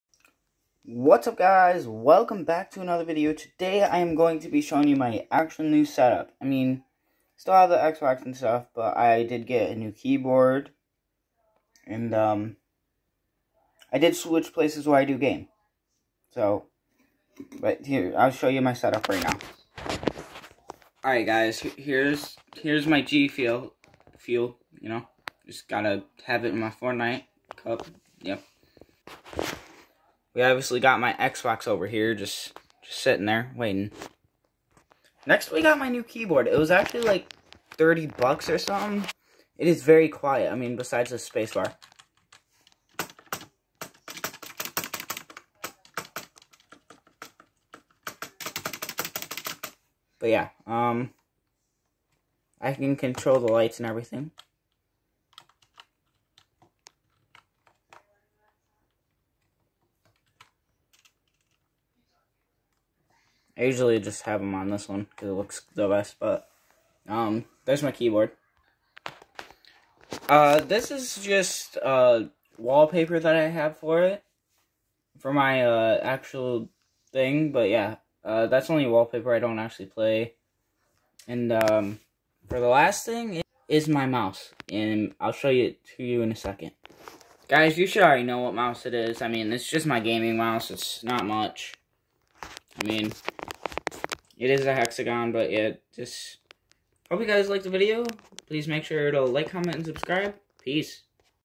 MY NEW SETUP???????
Today guys I will be showing you some new things i added to my gaming setup